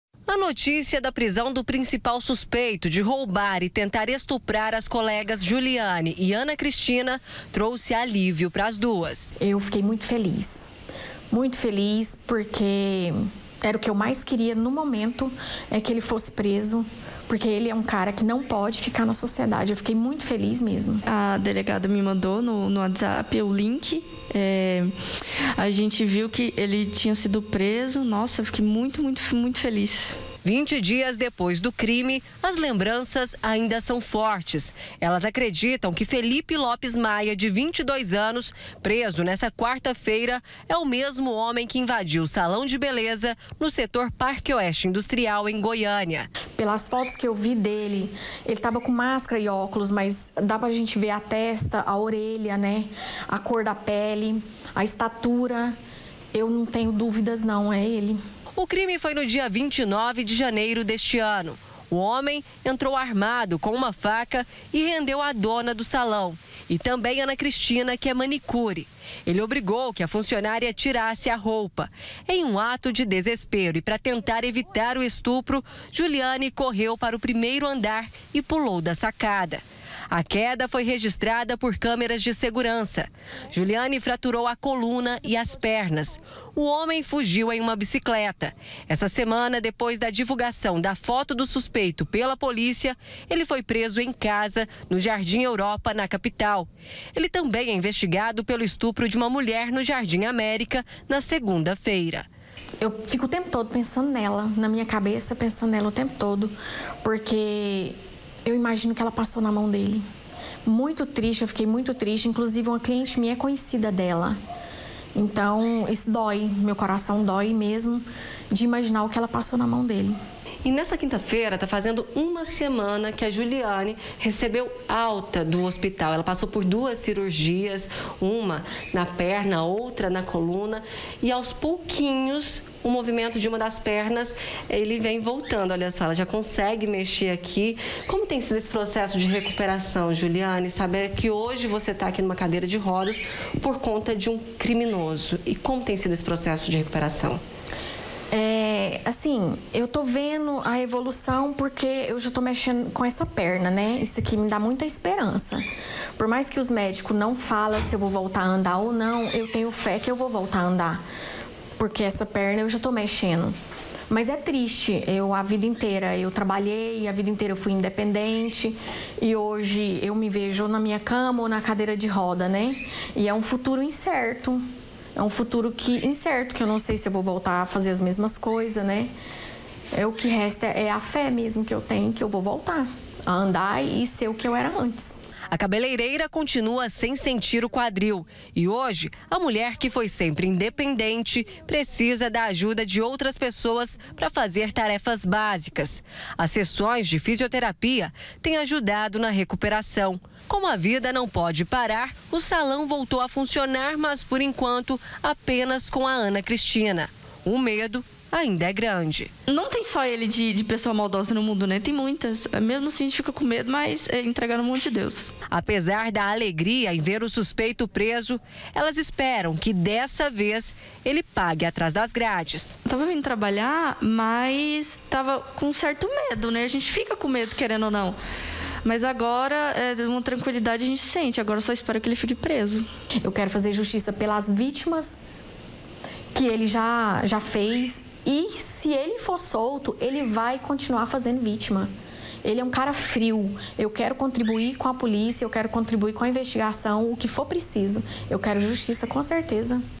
NA CADEIA: VÍTIMAS ALIVIADAS COM A PRISÃO DO TARADO
A DONA DO SALÃO QUE PULOU DO PRIMEIRO ANDAR PARA FUGIR DE UM ESTUPRO NÃO TEM DÚVIDAS QUE O HOMEM PRESO É O MESMO QUE INVADIU O COMÉRCIO DELA.